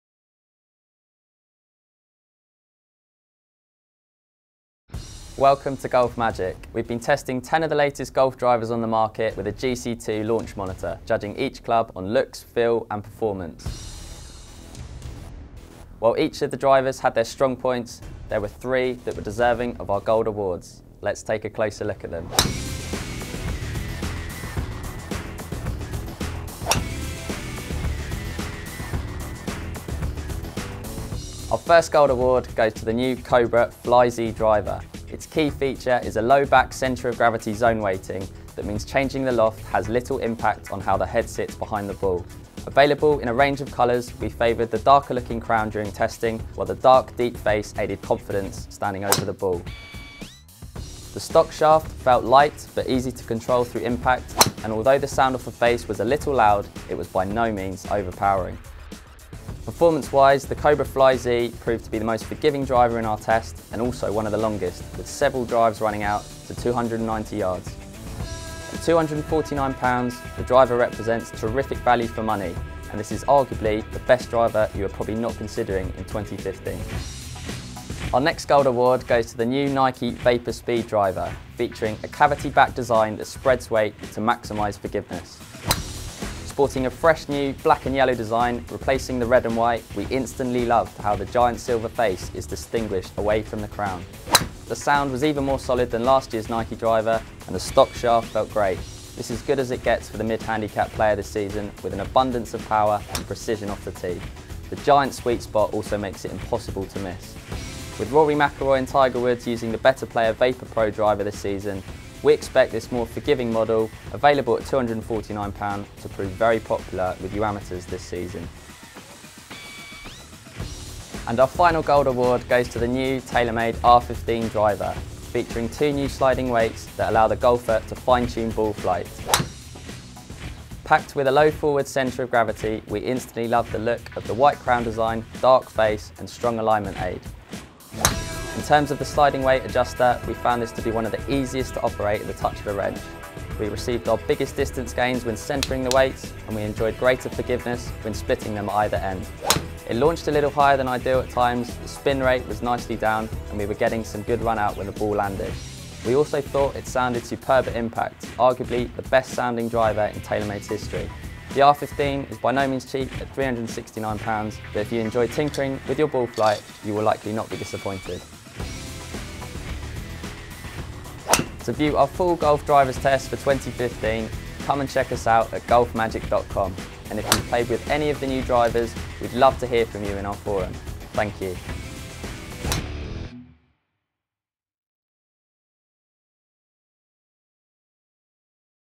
Best golf drivers on the market review 2015 | GolfMagic Club Test
Best golf drivers on the market tested and reviewed by GolfMagic.

We took 10 of the best of the current crop from Bridgestone, Callaway, Cobra, John Letters, Mizuno, Nike, TaylorMade and Wilson Staff and gave them a thorough workout.

Some add distance, others split fairways, a few look outright cool.

Several do it all. Have you hit any of these? What do you think of our verdict?